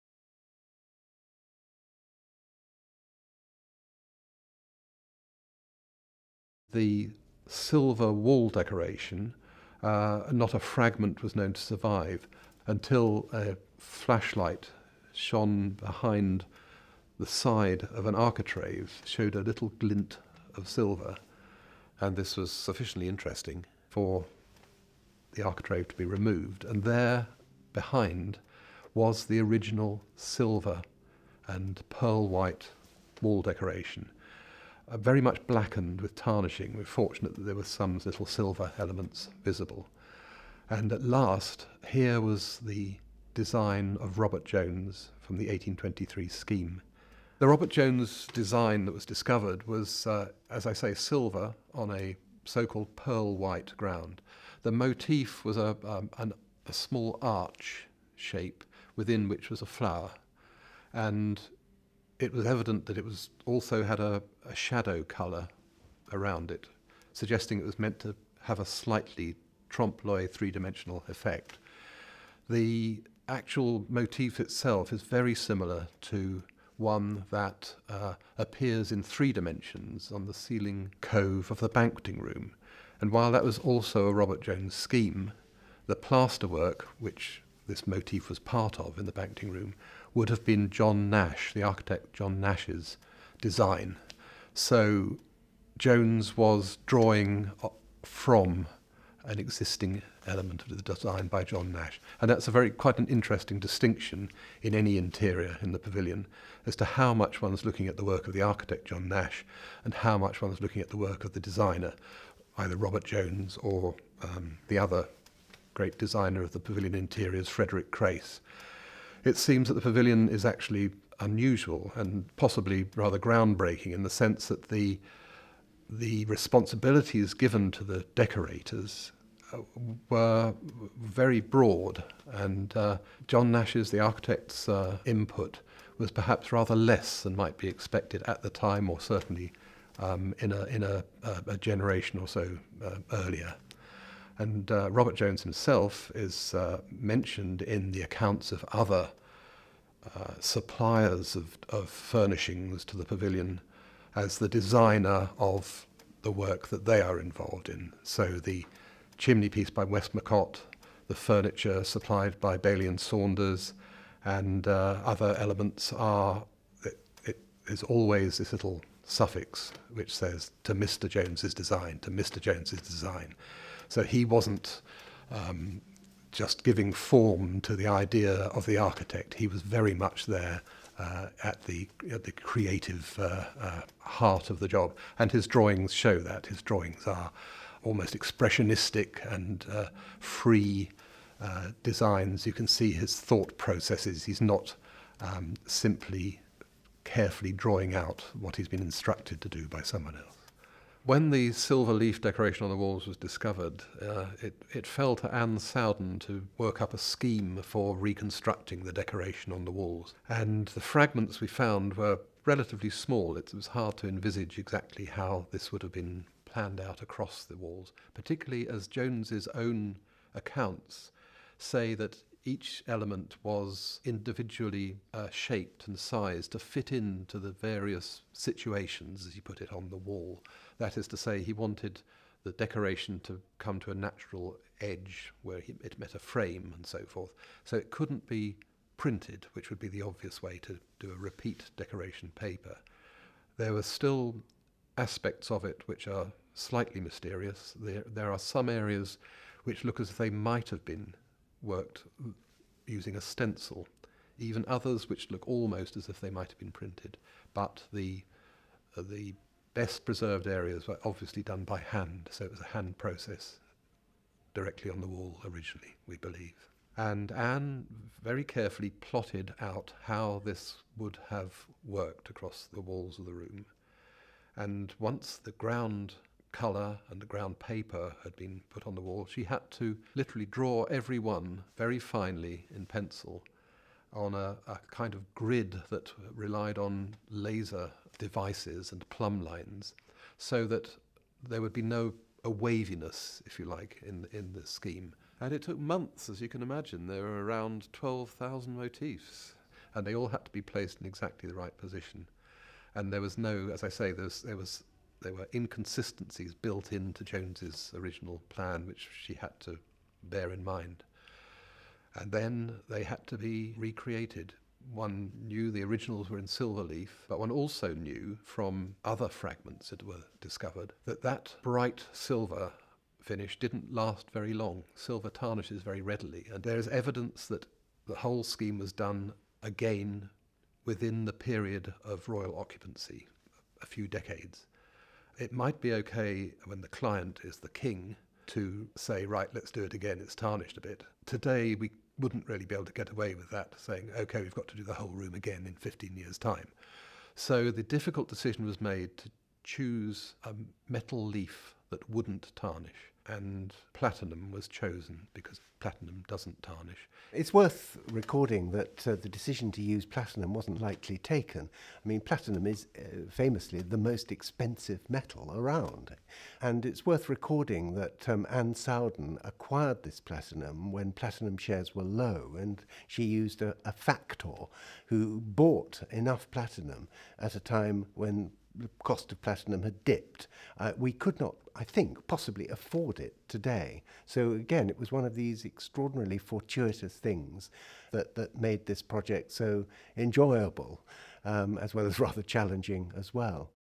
The Robert Jones Wall Decoration
A look into the discovery of the Robert Jones wall decoration in the Saloon in the Royal Pavilion and Ann Sowden's efforts to recreate the original design.